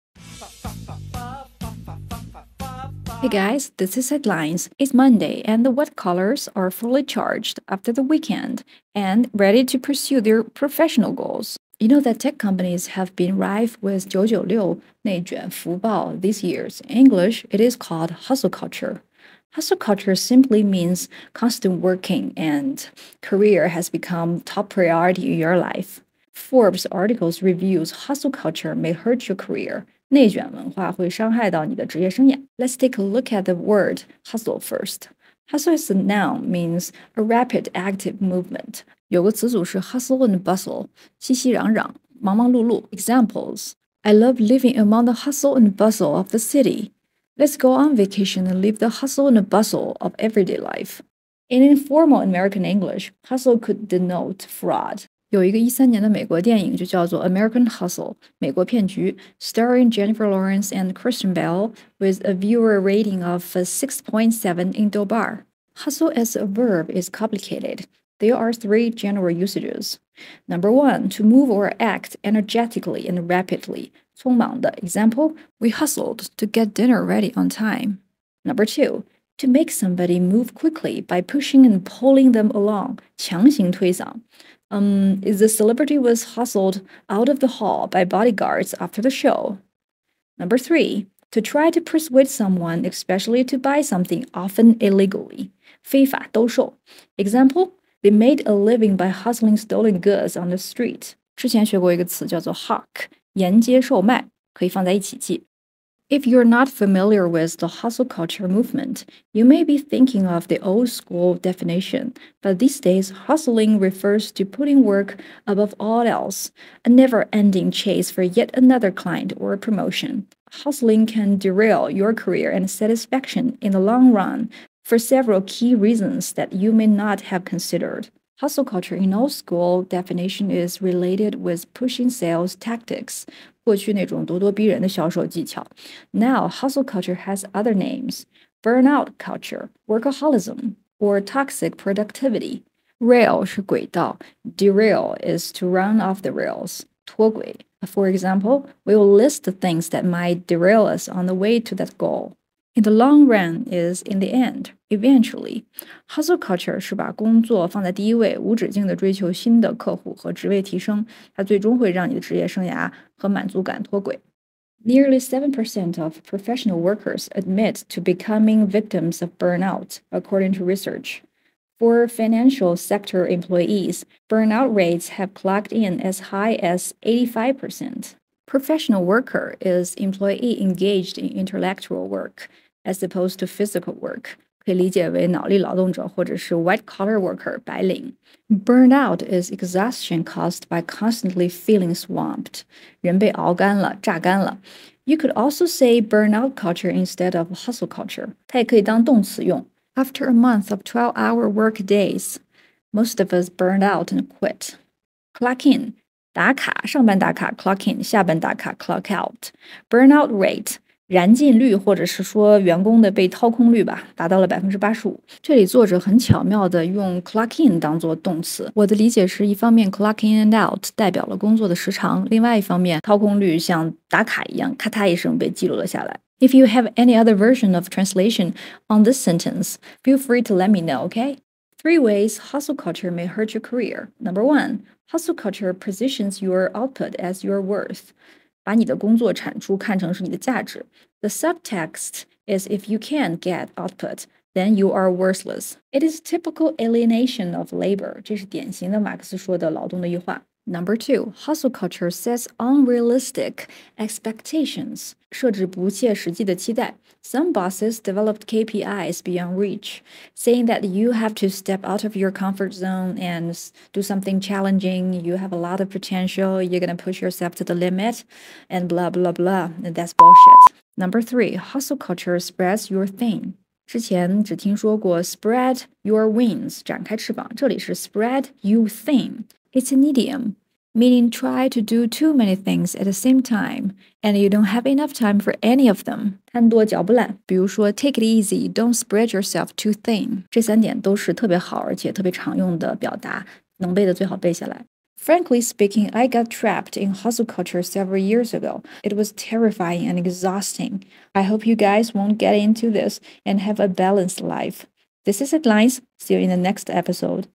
短新闻泛读 | Day 46：内卷文化会伤害职业生涯，原因有三点
个人学习的内容输出，不逐字逐句讲解翻译，只讲我认为重要或有讨论必要的~~
B站：英文标题党
wx公号：英文标题党
个人交流wx号：zoe_headlines